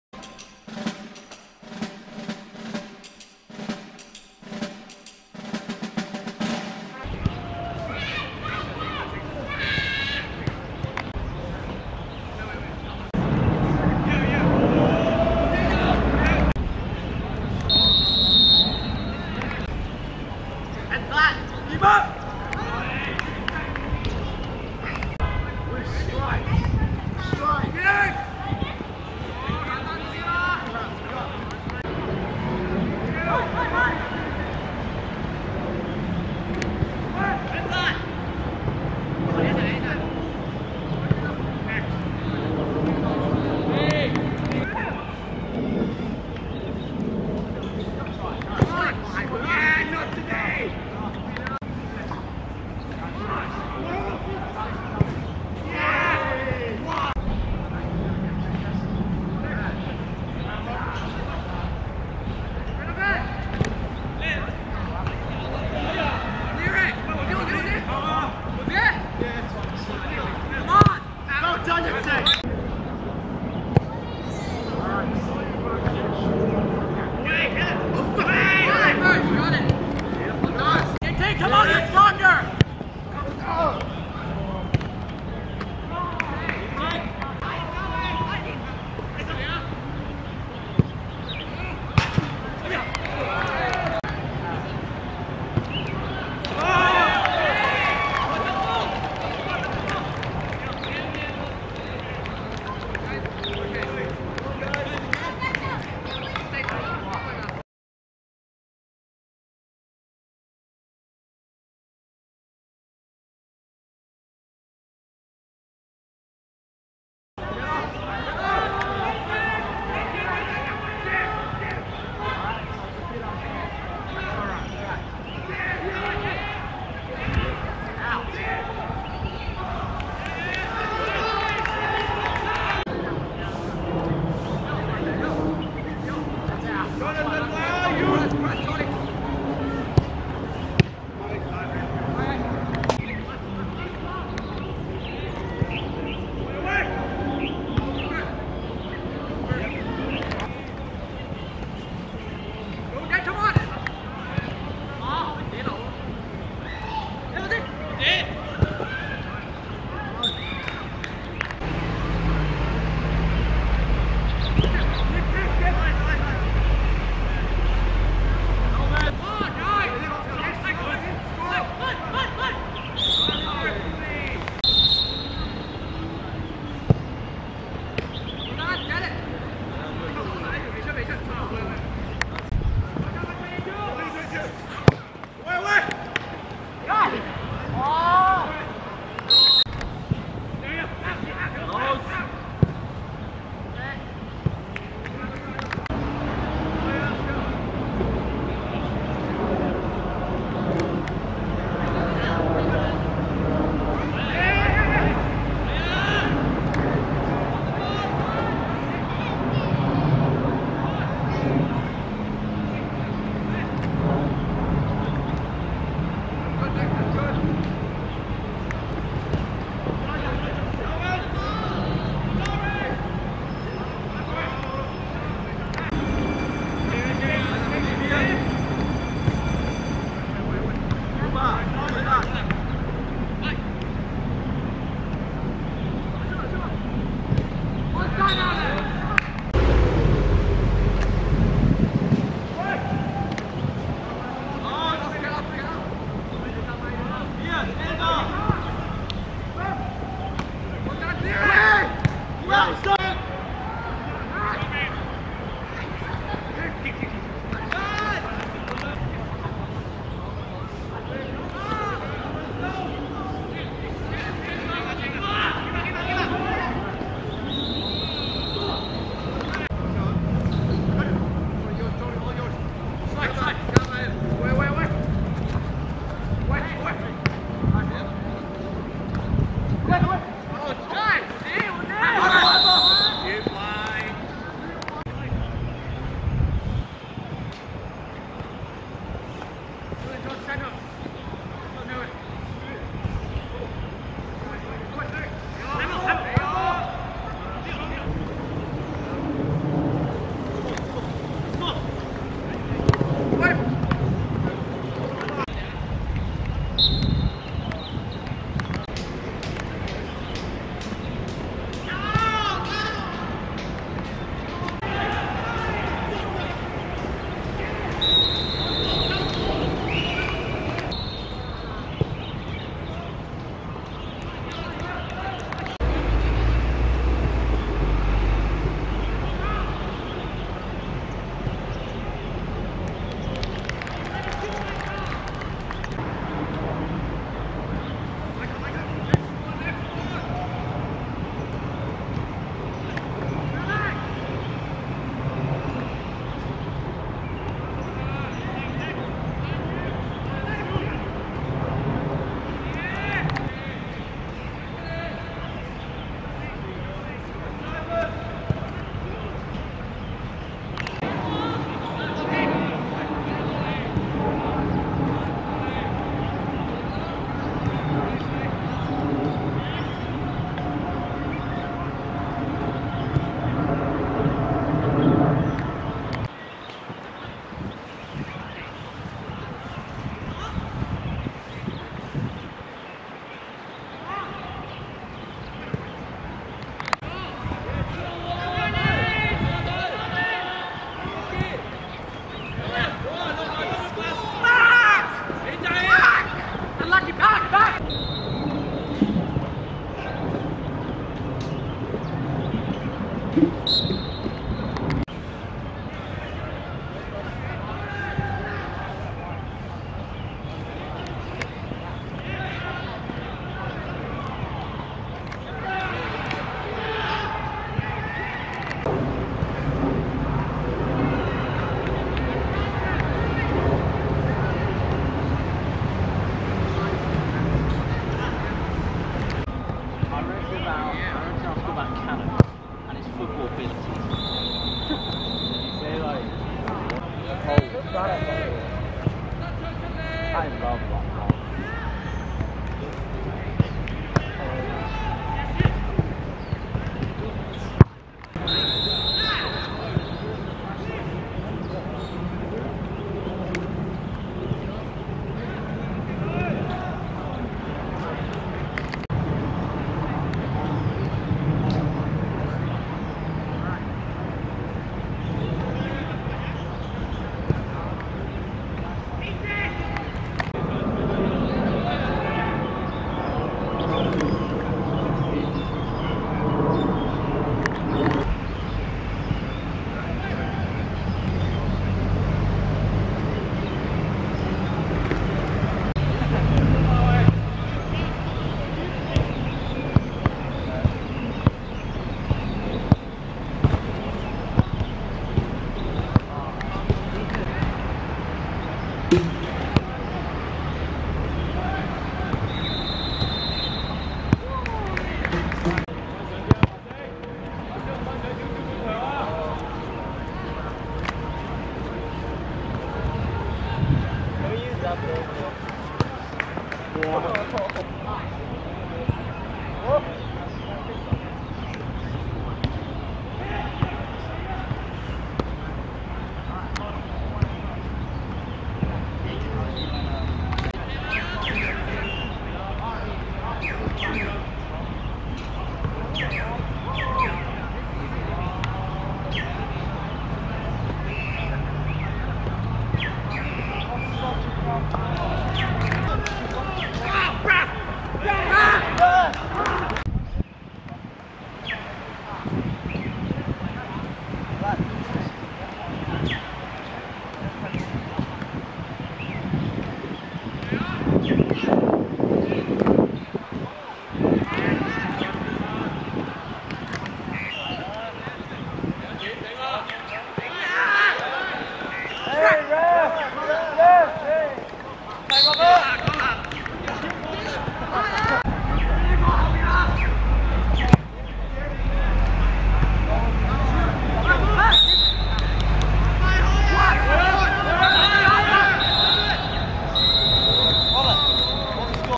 聖若瑟vs德瑞(2017.11.5.D1學界足球甲組)精華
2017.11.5.掃桿埔球塲 09:30 聖若瑟(白) 1 : 0 德瑞(綠)